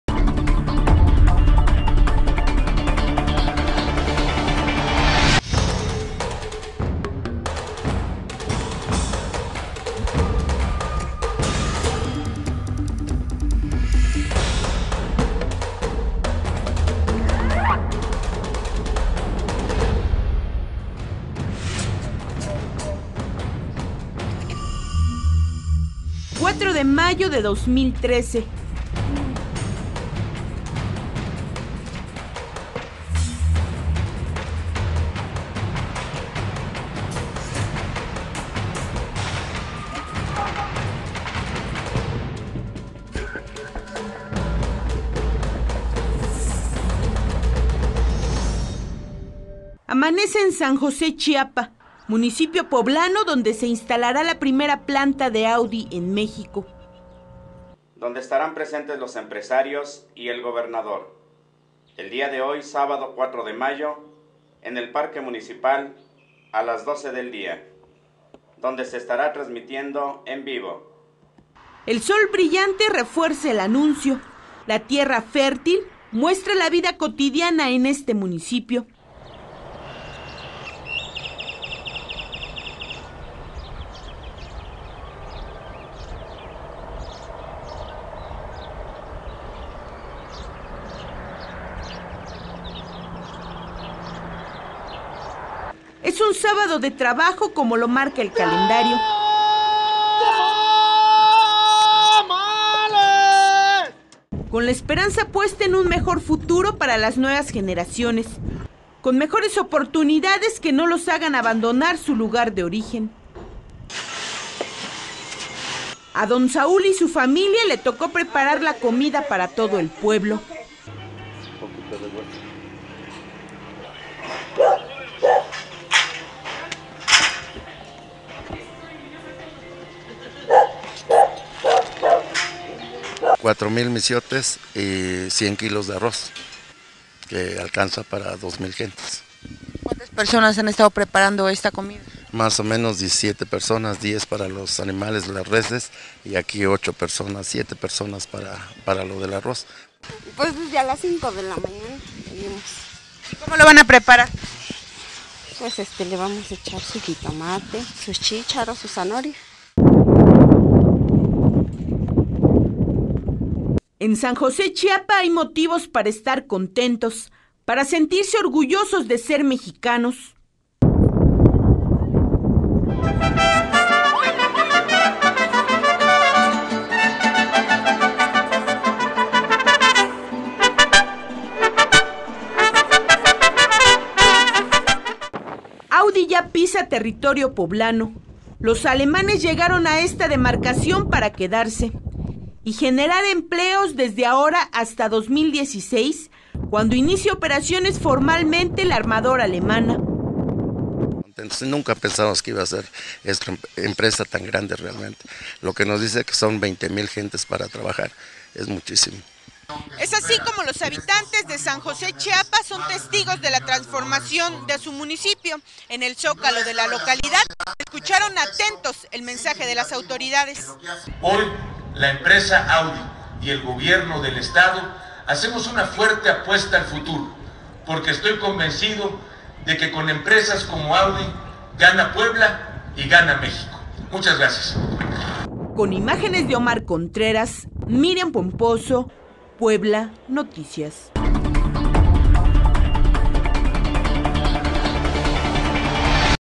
Audi marca un nuevo comienzo en San José Chiapa
Los habitantes recibieron con gusto la llegada de Audi, pues representa para ellos una gran fuente de empleo